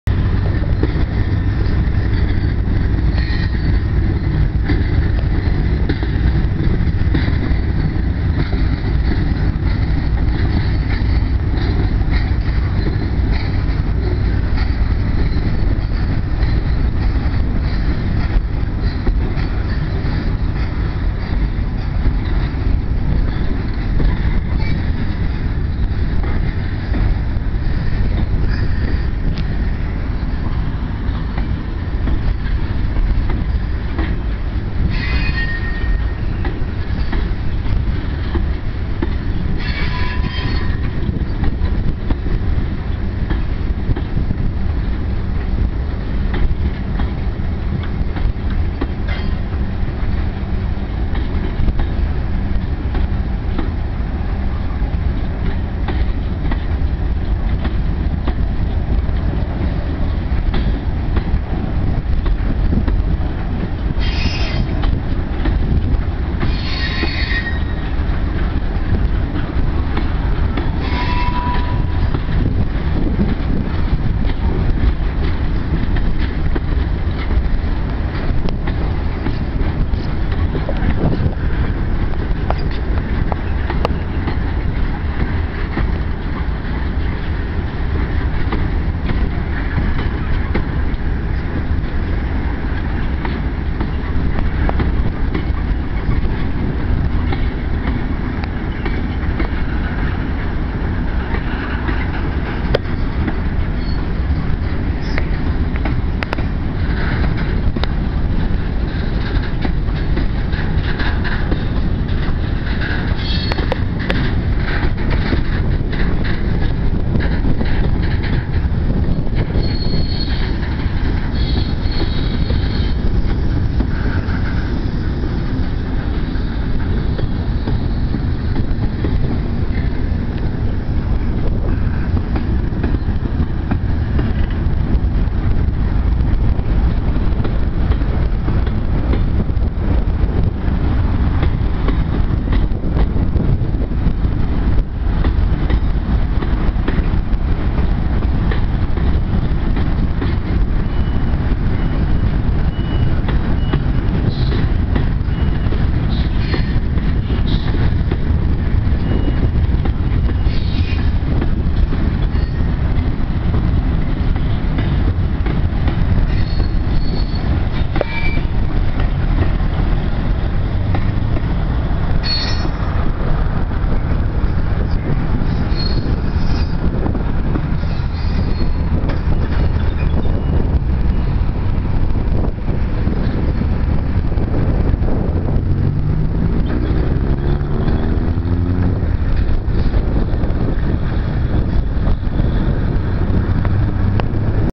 Tren cargado de NCA con la EMD GT-22CW 9042 saliendo hacia Rosario
En las últimas horas del dia y luego de detenerse 15 minutos para relevo de personal. Ahora la EMD GT-22CW 9042 mas un mediano tren cargado compuesto por 32 tolvas graneras procedentes de General Cabrera y 16 portacontenedores procedentes de Las Perdices acelera en el Paso a Nivel de la Ruta Provincial 12, ubicado a 3 kms al este de la Ciudad de Marcos Juárez, para ya salir rumbo a Rosario. Cercanias de Marcos Juárez. Córdoba. Argentina. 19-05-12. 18:02 hs.